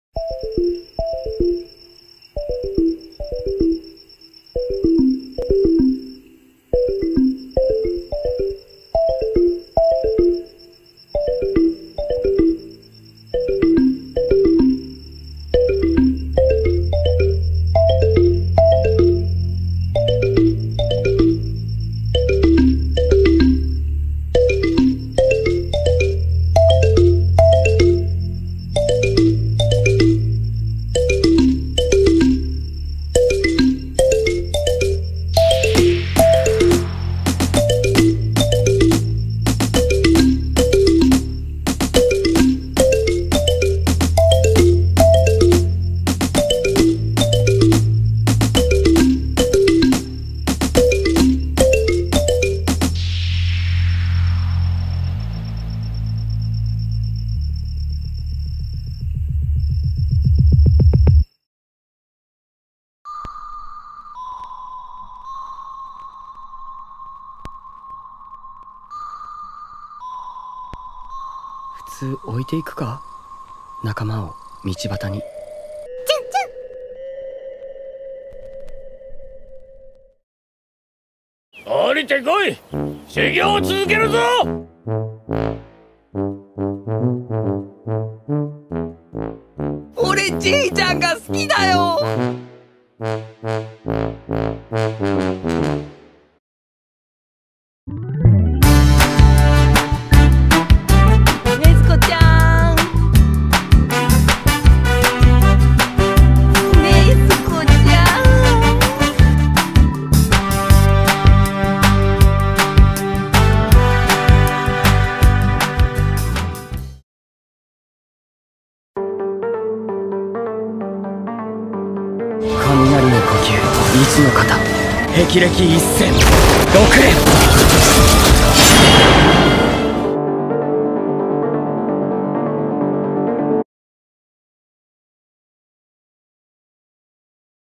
귀멸의 칼날 젠이츠 피규어 언박싱: 징징빌런을 극복하라 | Demon Slayer Zenitsu Figure Unboxing | with 홈가드닝(Home Gardening)
일본내 귀멸의 칼날 캐릭터 인기순위 1위
(극복하고나면 끄덕끄덕)